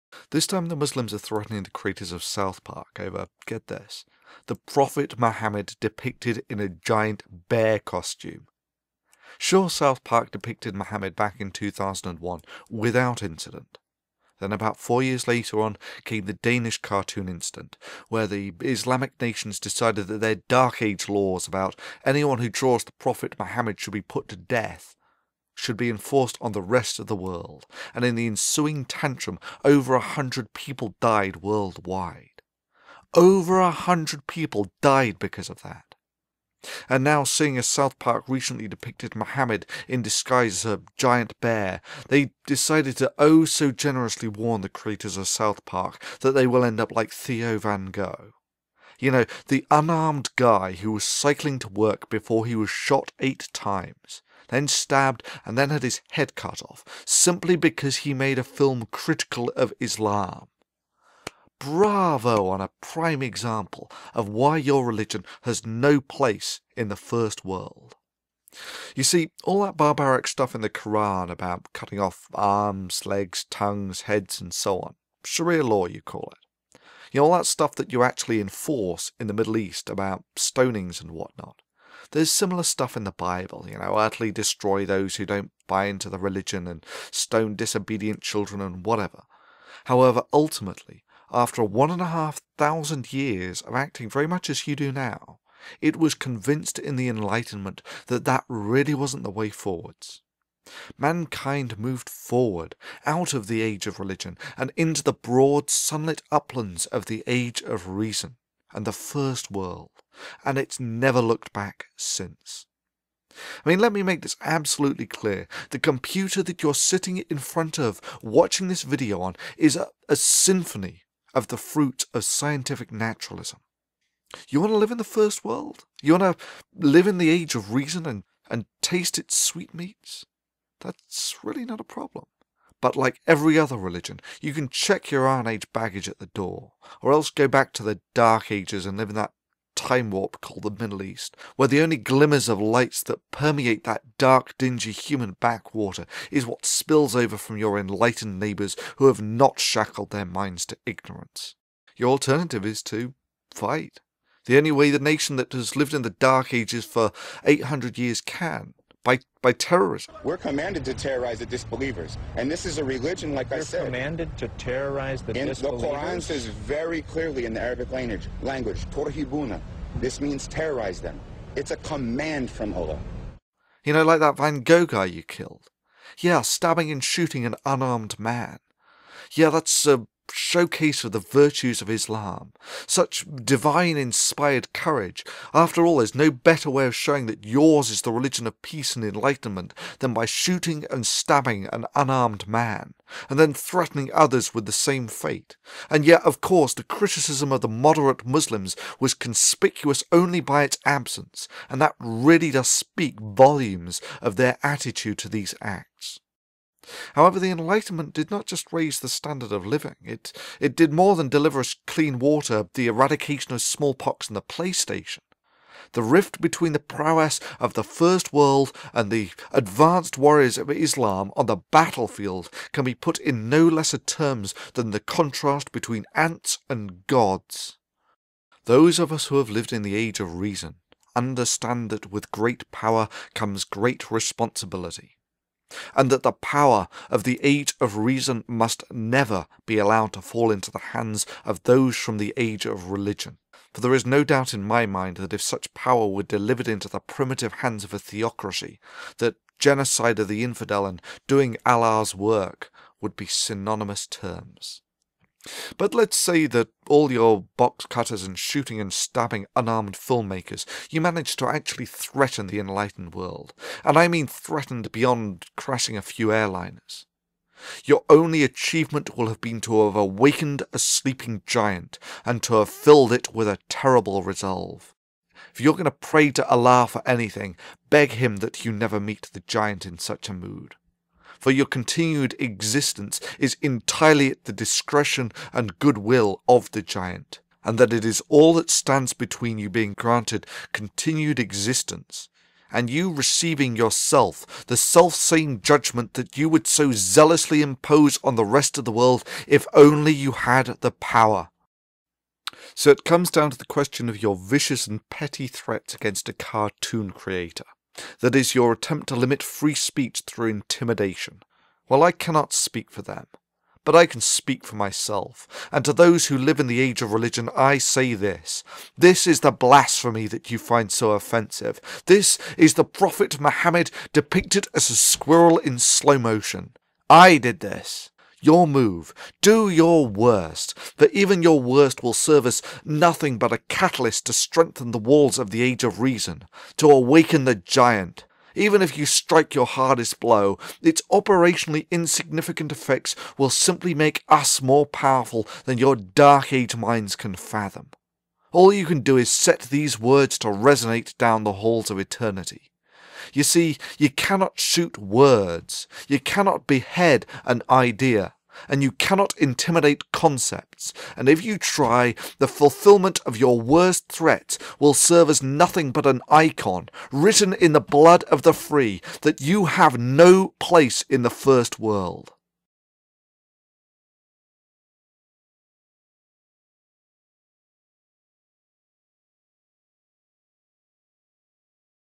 South Park vs Islam
So the US-based Muslim website Revolutionmuslim.com posted a barely concealed veiled threat against the creators of South Park, Trey Parker and Matt Stone for depicting the Prophet Muhammad in a bear costume.

Well kudos to the South Park creators for showing the backbone to defiantly stand against those who hope, through threats of violence, to have a chilling effect on free speech.

I see no reason to mince words here.  Theo van Gogh was killed for nothing other than making a film critical of Islam.  He was shot, stabbed and beheaded in the capital of a First World country.  Over a hundred were killed world wide in riots etc over the Danish cartoons.
-It is unacceptable, unthinkable, that we should stand by and allow such folk to import the core of their religion, -the core which has kept their civilization in the dark ages for the last 800 years into our enlightened civilization.  Not while I have my strength.  I have seen the fruits of the Enlightenment, and I have seen the stagnation of the Middle East.
We cannot allow them to do this as firstly they do not have the maturity to wield the power of the Age of Reason, and secondly who in their right might would wish to re-embrace the Dark Ages.

Sharia Law is an irrelevance in the first world, and if you think that if someone draws a picture of your 'Prophet' that this means you have the right to kill them, then you have no place in the First World.